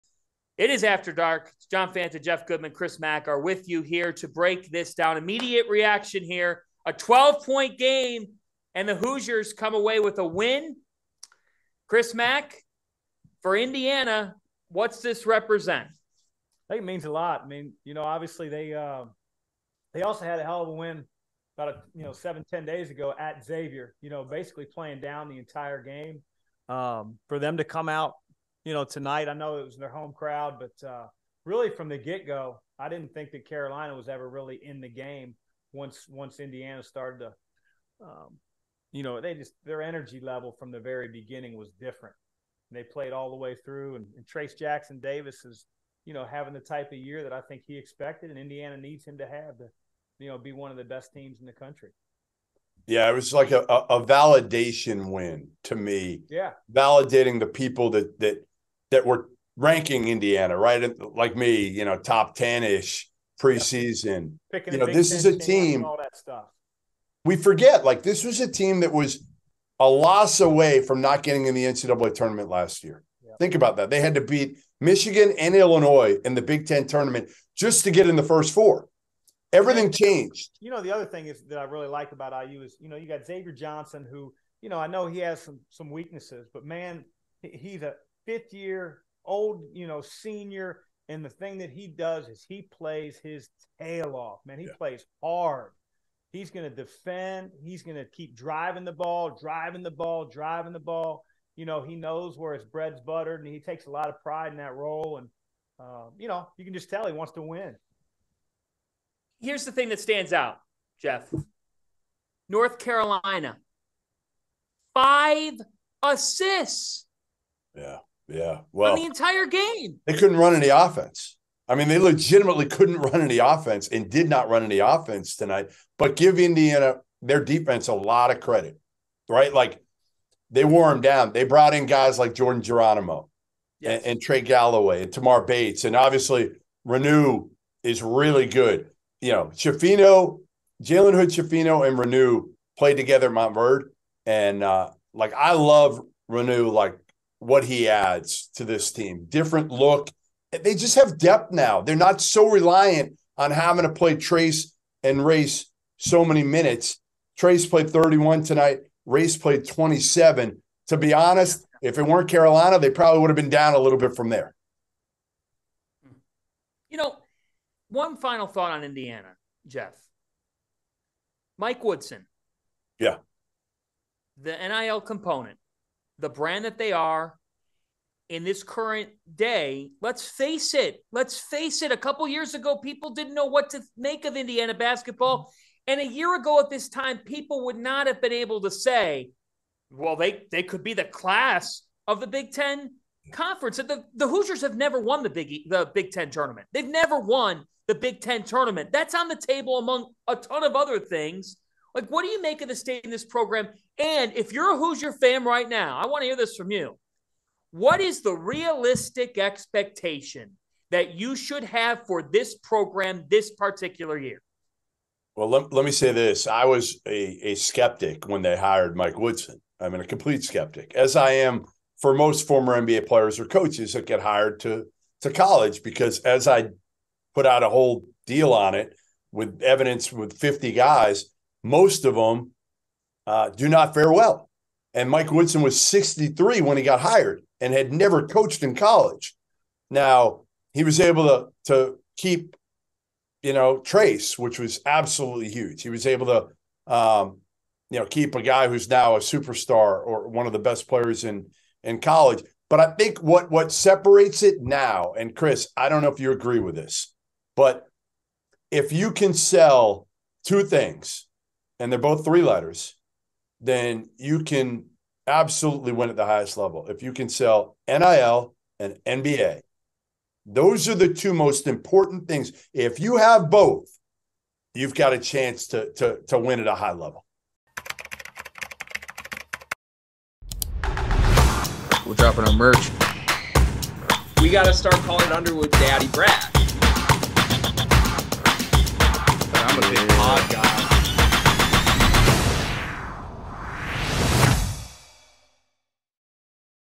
Indiana proved why they’re a LEGIT top 10 team against North Carolina!! | AFTER DARK
The Hoosiers are LEGIT after their big win over North Carolina!!